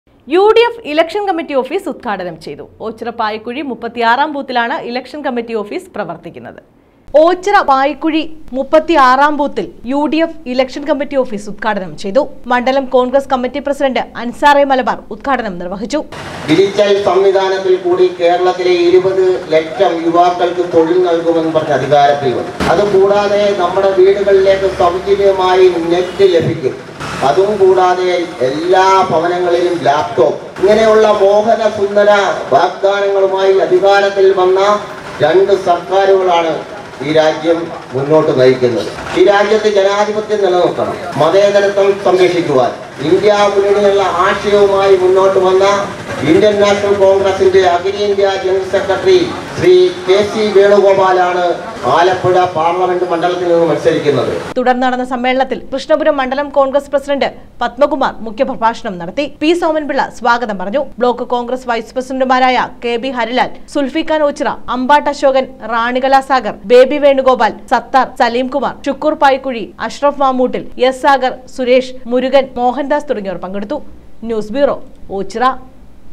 യു ഡി എഫ് ഇലക്ഷന്‍ കമ്മറ്റി ഓഫീസ് ഉദ്ഘാടനം ചെയ്തു.
യു ഡി എഫ് ഇലക്ഷന്‍ കമ്മറ്റി ഓഫീസ് ഉദ്ഘാടനം ചെയ്തു. ഓച്ചിറ പായിക്കുഴി 36-ാം ബൂത്തിലാണ് ഇലക്ഷന്‍ കമ്മറ്റി ഓഫീസ് പ്രവര്‍ത്തിക്കുന്നത്.